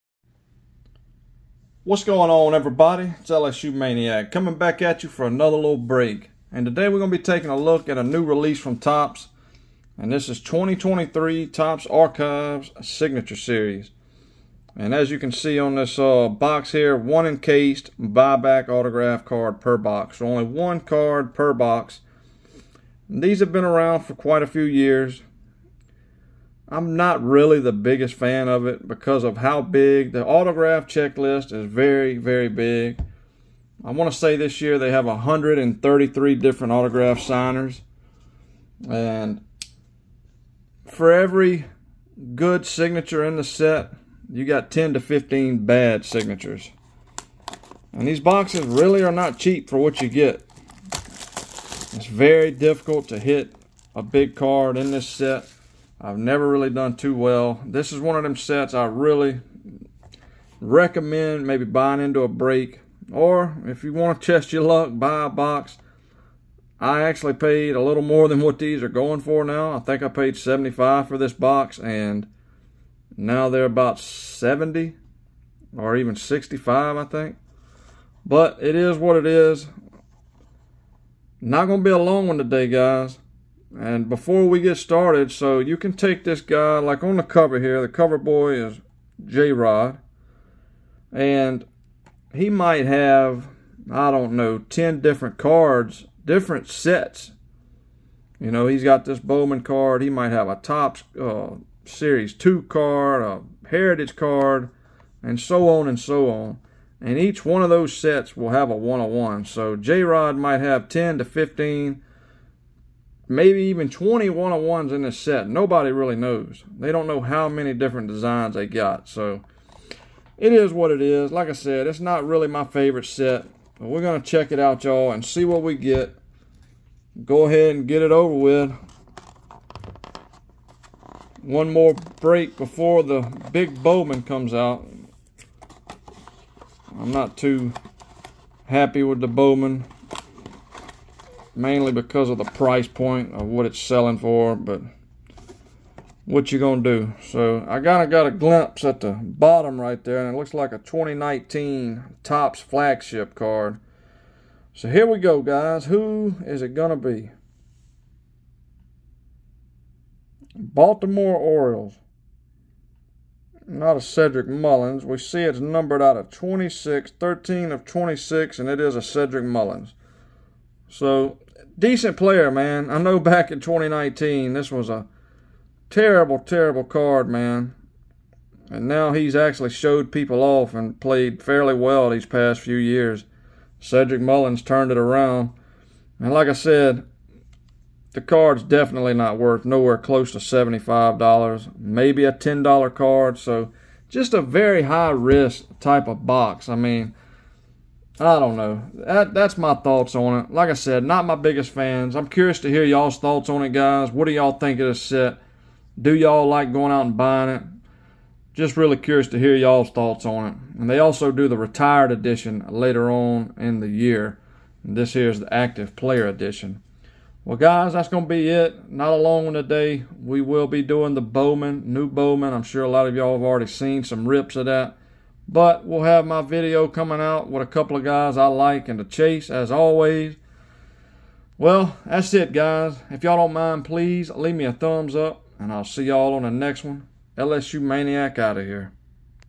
NEW RELEASE!!! 2023 Topps Archives Signature Series Active Player.
In today's video we are ripping the new release from topps 23 archives signature series active player. This set features one buyback card per box and with 133 different signers it makes it very difficult to hit the big names. These will run around $70 and in my opinion is steep for what you get. Anyway let me know what yall think of this one and as always Happy Collecting!!!